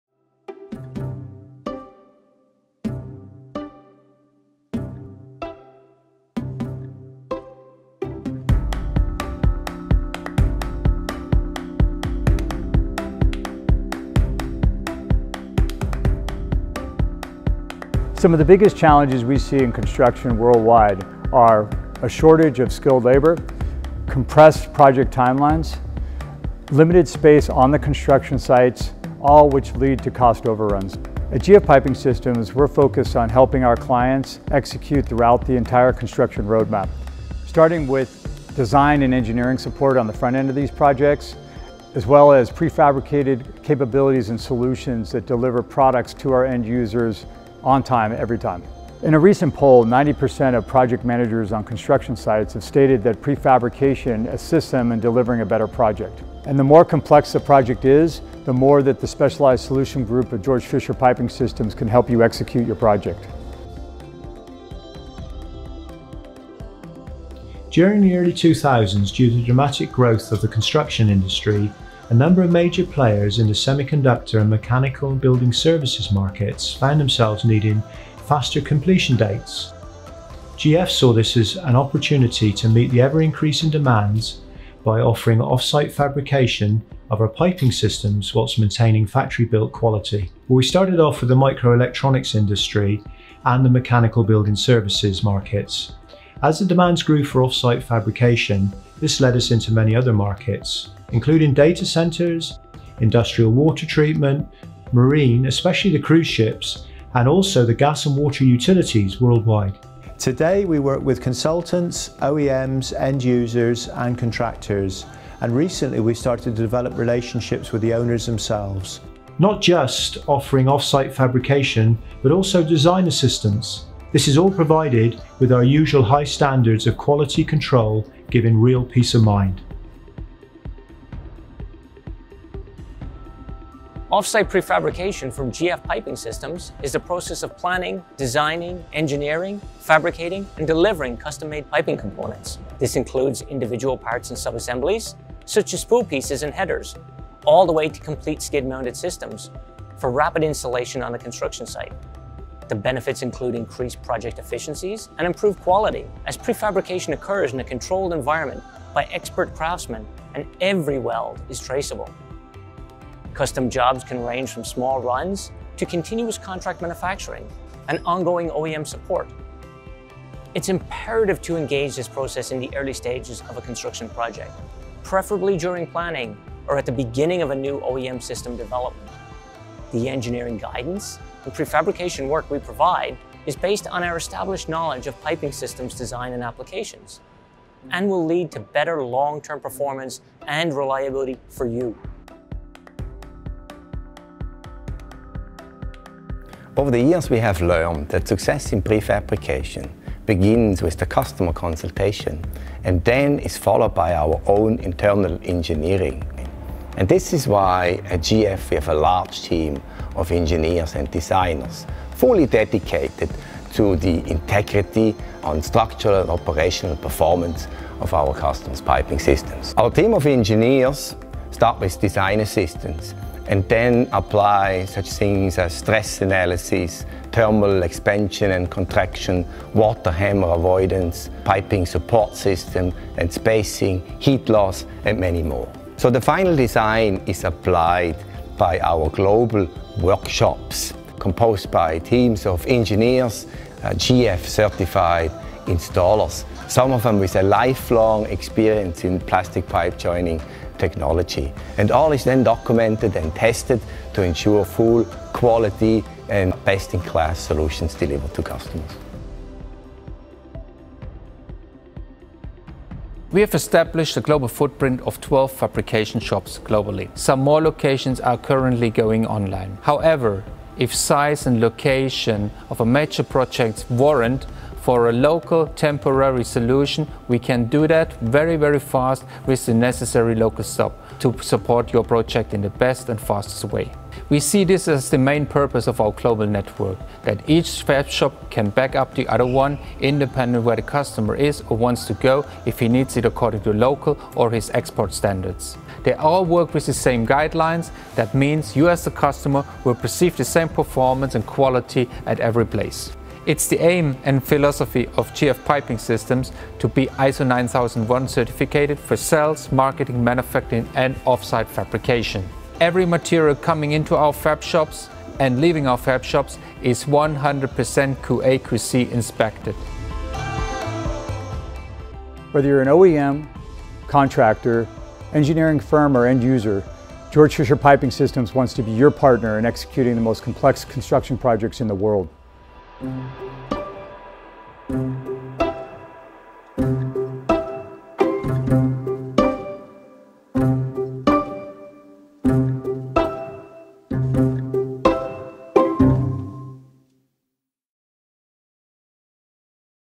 Engineering & Prefabrication with GF Piping Systems: The Challenges You Face
Some of the biggest challenges in construction worldwide, like shortage of skilled labor and compressed timelines, lead to cost overruns.

At GF Piping Systems, we focus on helping our clients thoughout the entire construction roadmap, starting with engineering support as well as prefabrication capabilities and solutions that deliver products to our end users on time, every time.